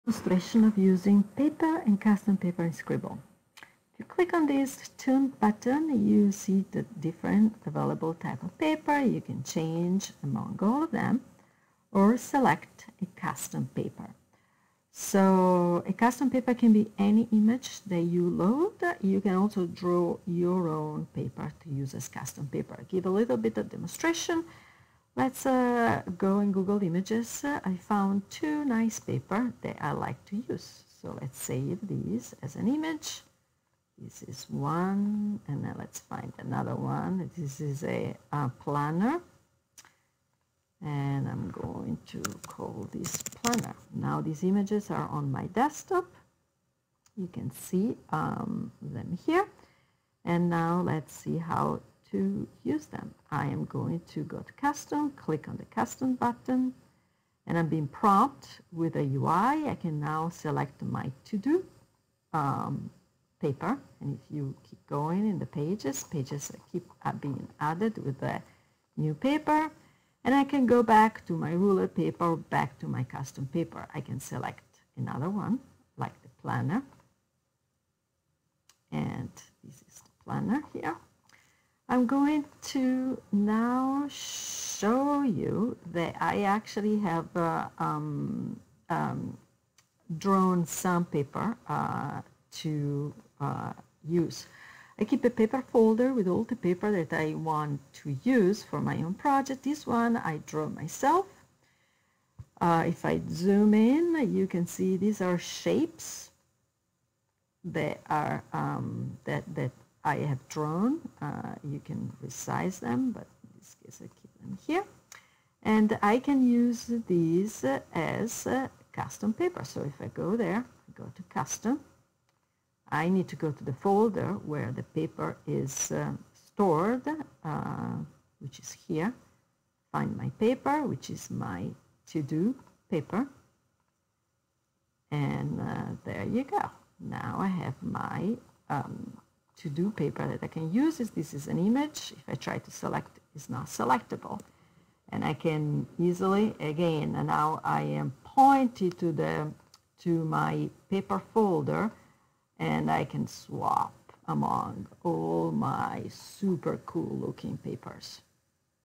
Custom Paper in Scrble Ink
This videos shows how to import custom paper, draw your own custom paper, and organize all your custom paper in a special folder to allow easy switch among different paper types